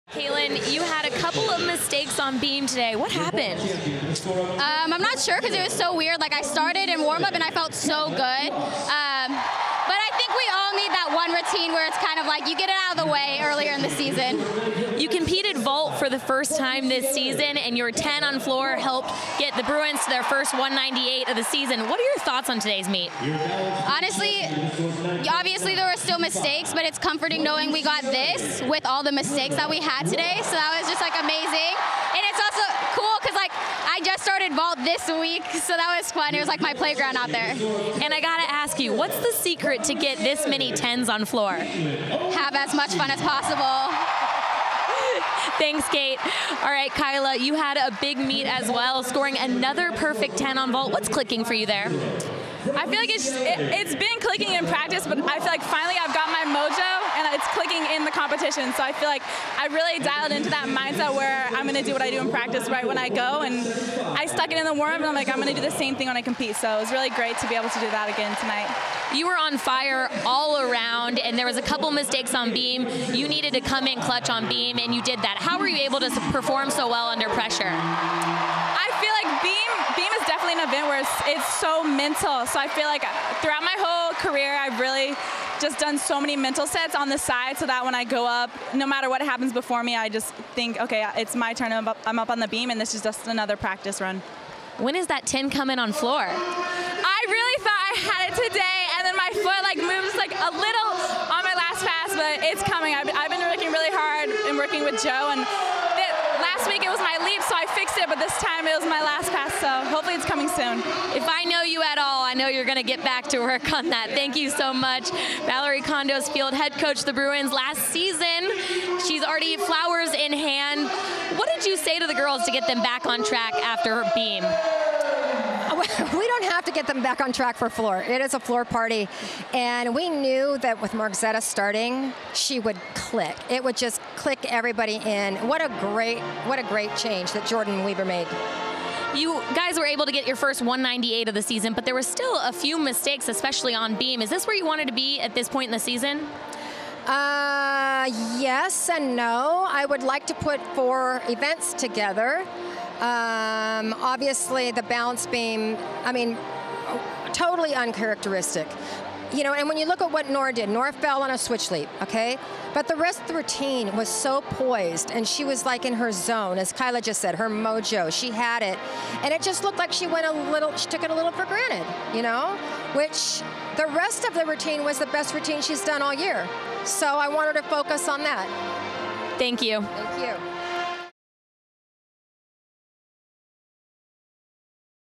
UCLA vs. Arizona Postmeet Interviews
Pac-12 Networks' Samantha Peszek catches up with UCLA gymnasts Katelyn Ohashi and Kyla Ross following the Bruins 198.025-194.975 win over Arizona in Los Angeles on Saturday. Ohashi finished with a perfect 10 on floor for her third overall perfect 10 of the season. Ross took home a perfect 10 of her own on vault and finished with an all-around NCAA season high of 39.850.